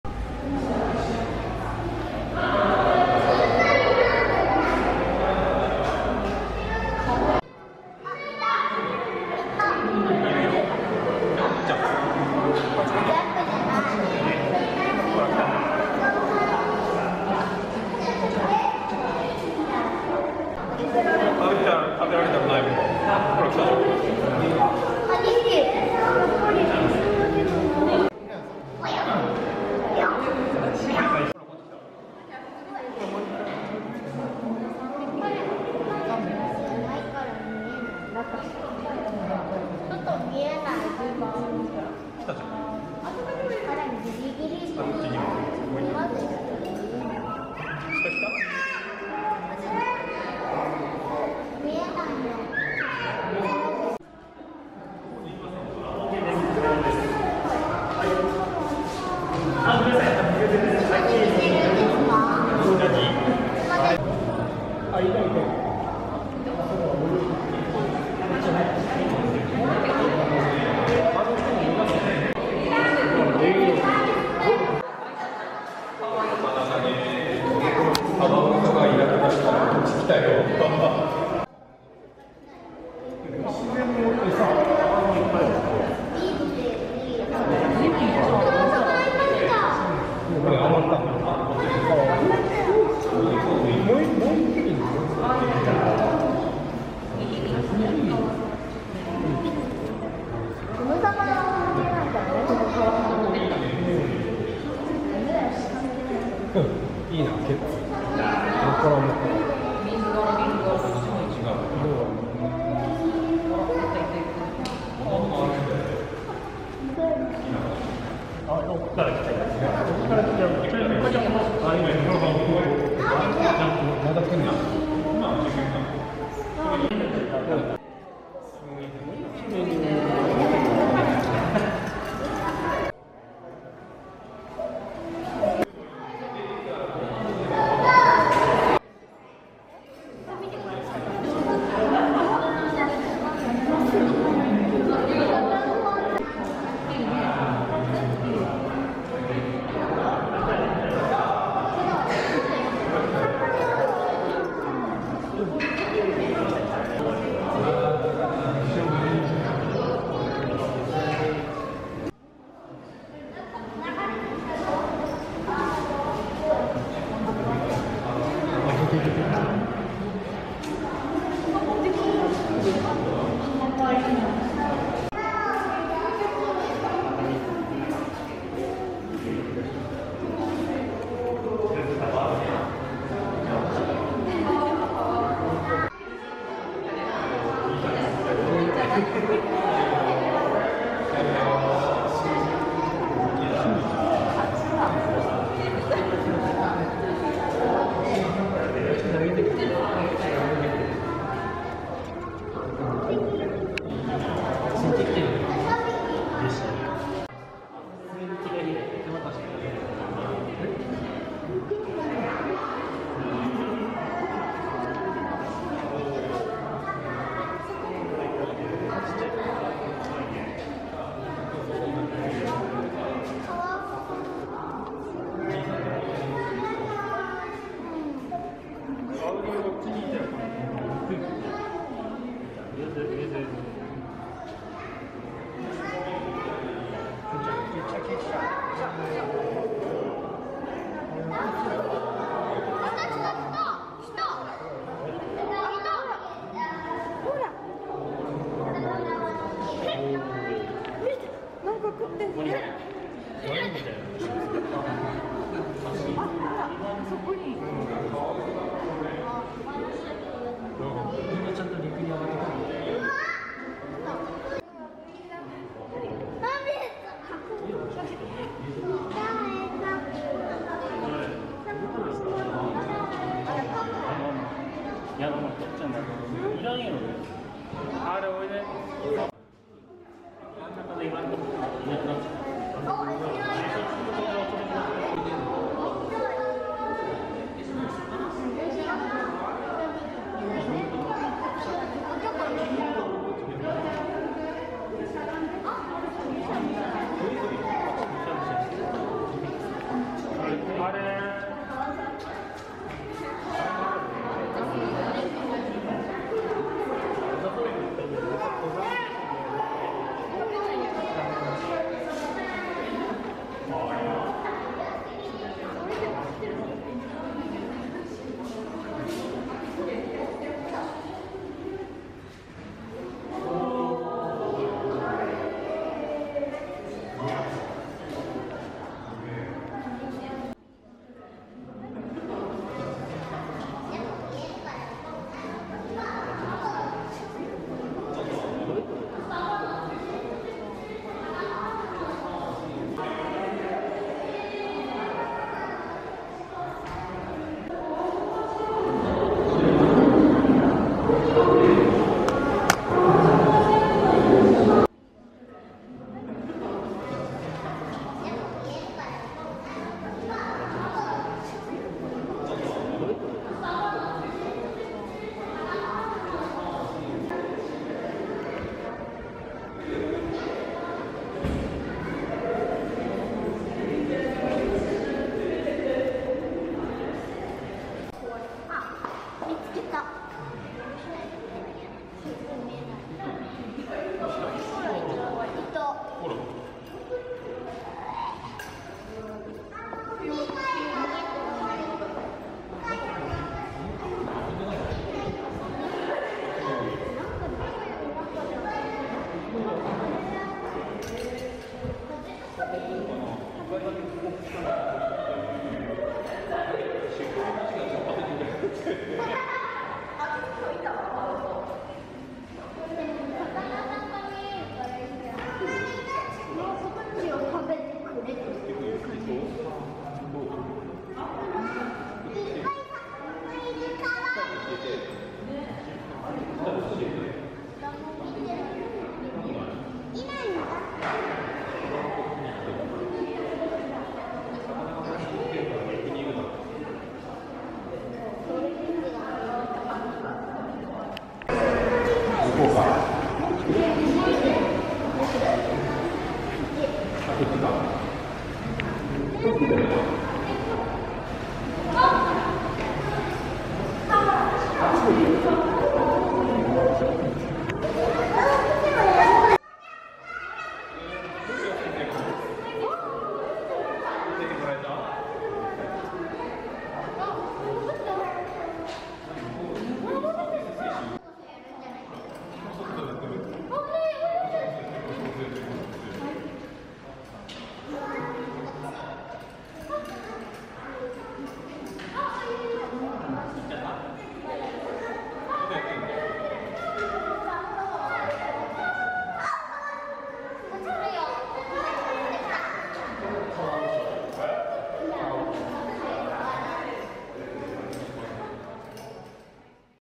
ユーラシアカワウソ
ユーラシアカワウソです。
：食肉目 イタチ科
：まろんちゃん、風（ふう）ちゃん、ヨシちゃん
：アクアマリンふくしま、日本
：2024年5月
：通常の生息地は、ユーラシア大陸など